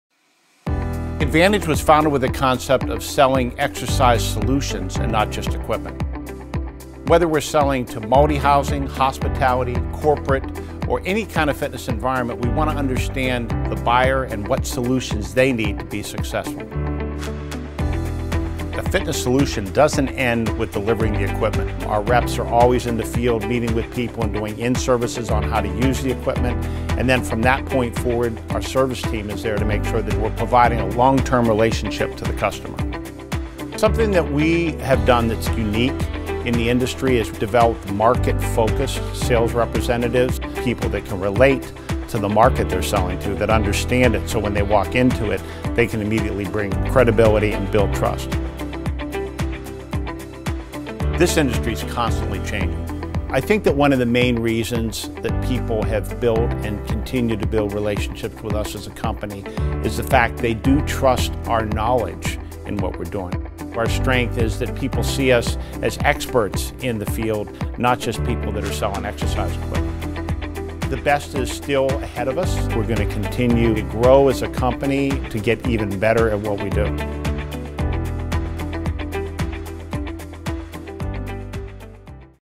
Inside Advantage Sport & Fitness
Since 1987, we've been partnering with clients to provide premium commercial fitness equipment and customized gym designs. Go behind the scenes to learn how we approach each project to design complete fitness solutions.